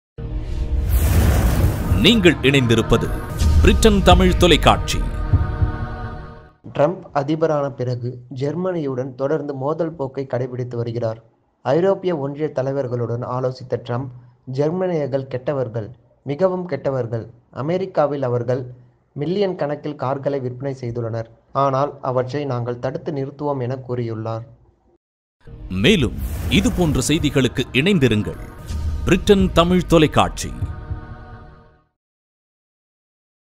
Trump against Germany.
Germany people sold many cars in USA so Trump decided to go against Germany. Please watch this video for detailed news.